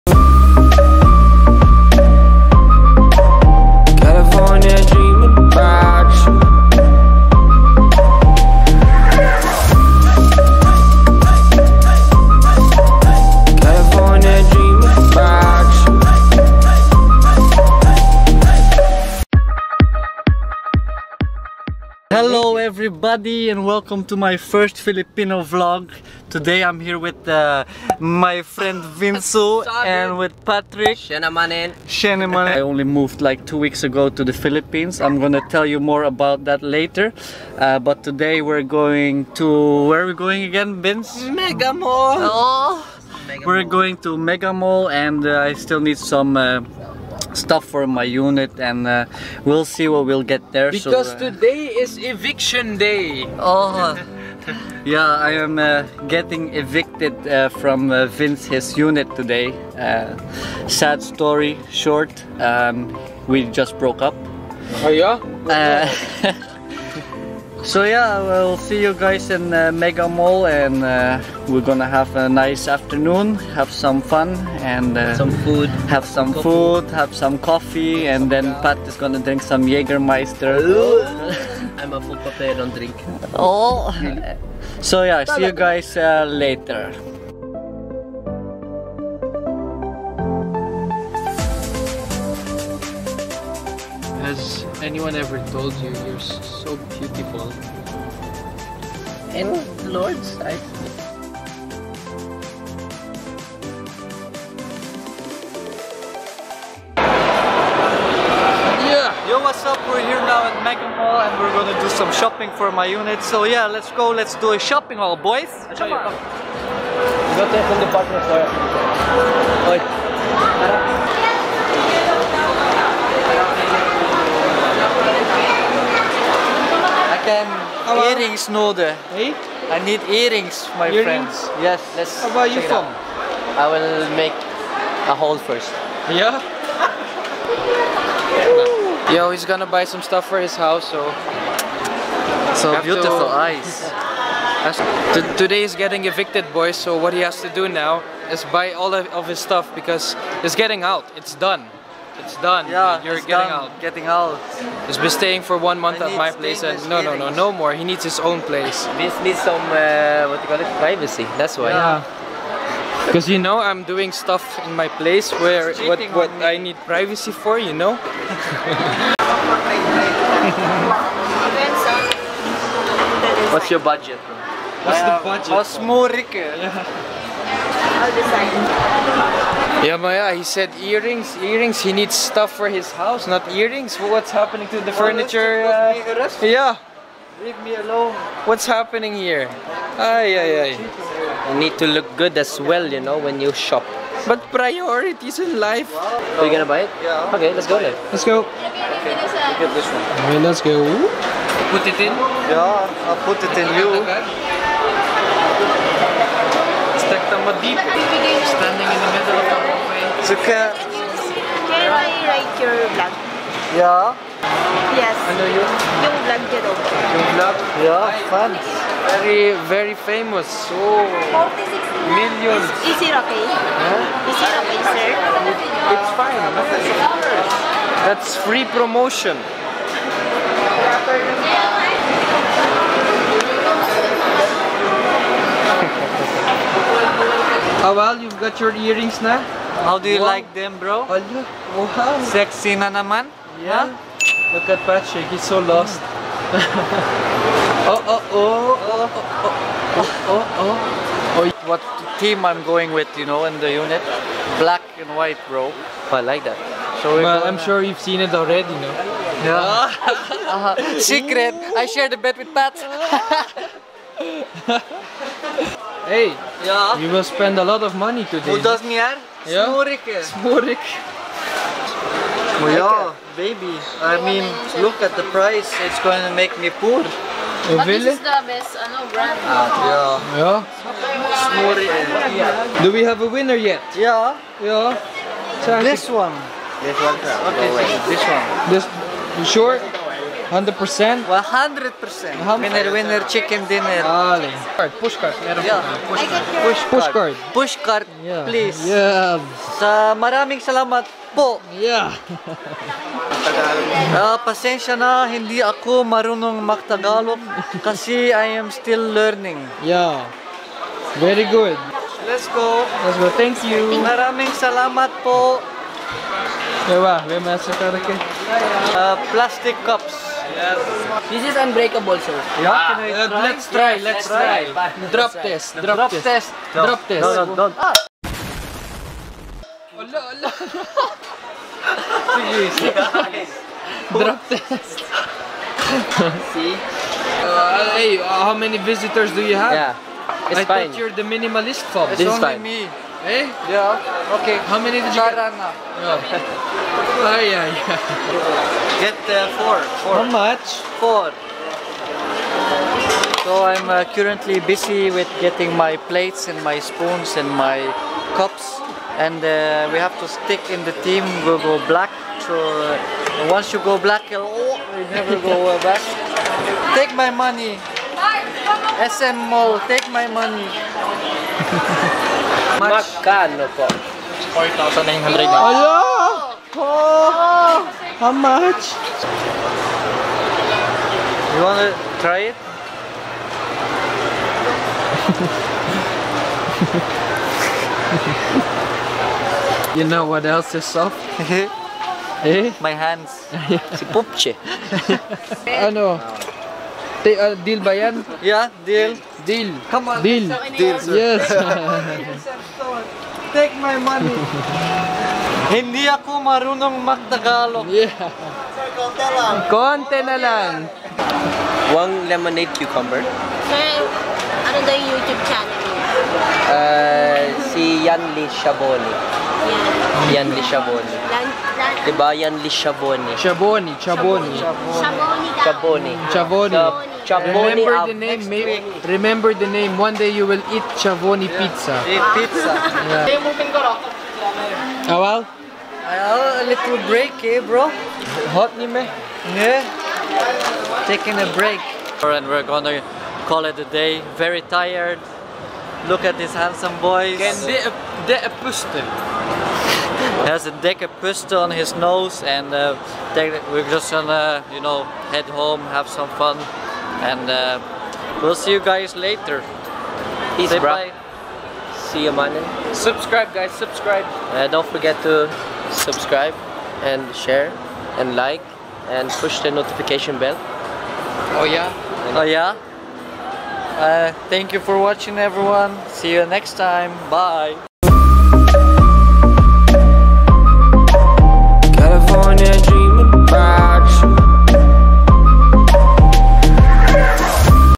I GOT EVICTED !! - Shopping with Vince vandorpy & Patrik
First Vlog made here, In the Philippines. Was not going to post it at first but then had a quick change of heart. Enjoy !!

Also Check out Vince Vandorpe his youtube channel and Patrik his social for more awesome content.